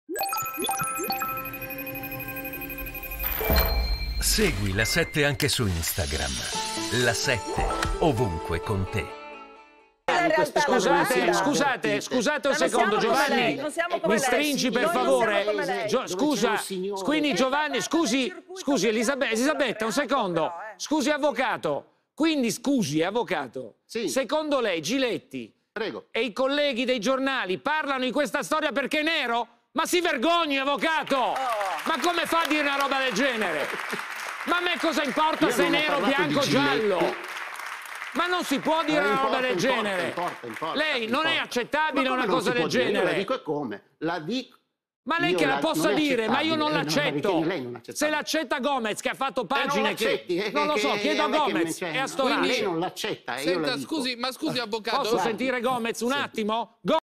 Soumahoro, Giletti contro l'avv. Prado: "Si vergogni! Cosa mi importa se è nero o giallo?"
Soumahoro, Massimo Giletti si scaglia contro l'avv. Prado: "Si vergogni! Cosa mi importa se è nero o giallo?"